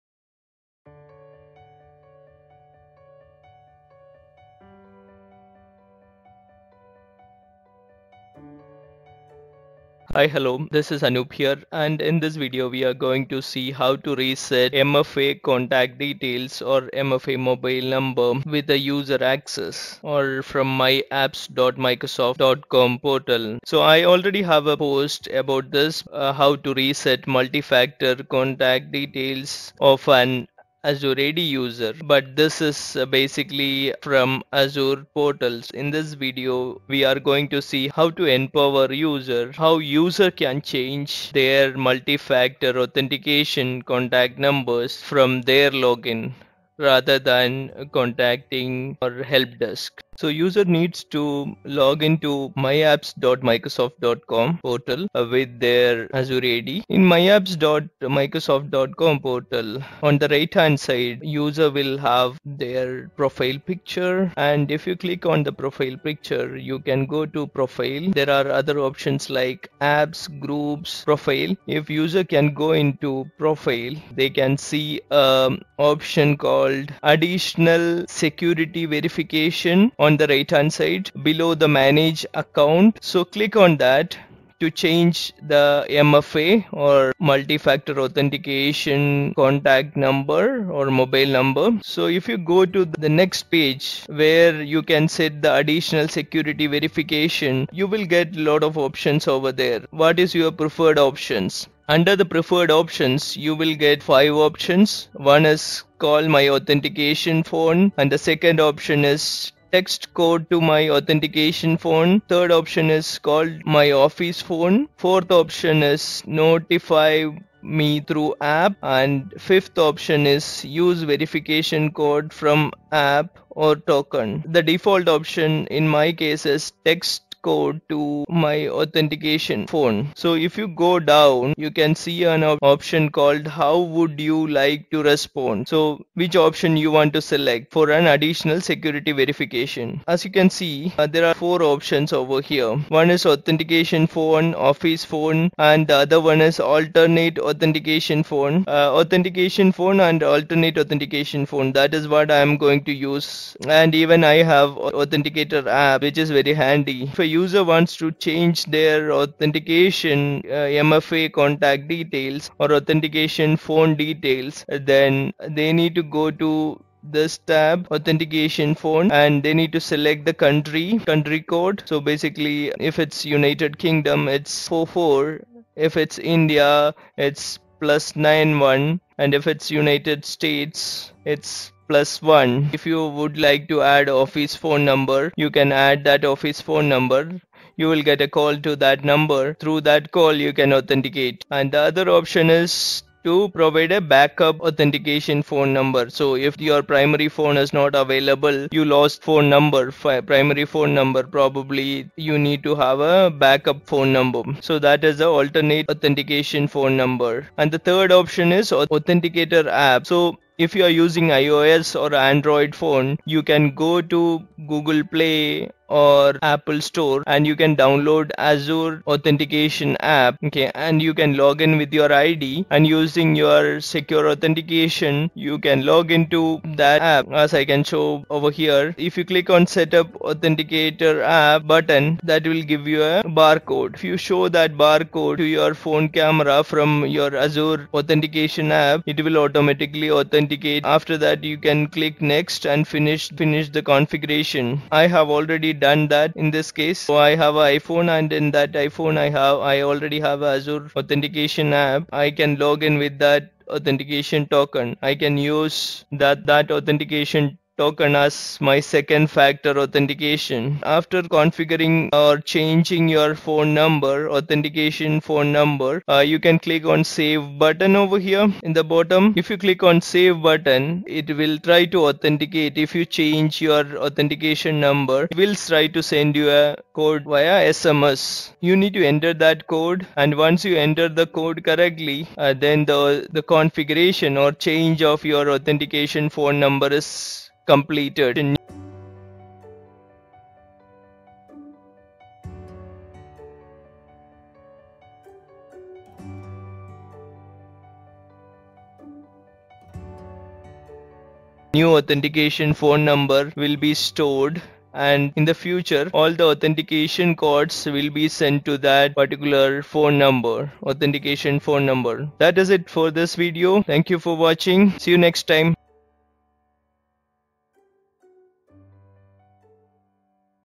Learn How to Change Azure Authentication Phone Number from MyApps Portal using End User Access
Change MFA Authentication Phone Number from MyApps Portal. In this video you will learn how to change AzureAuthentication Phone Number ! MFA Multi Factor Authentication Phone Number. More details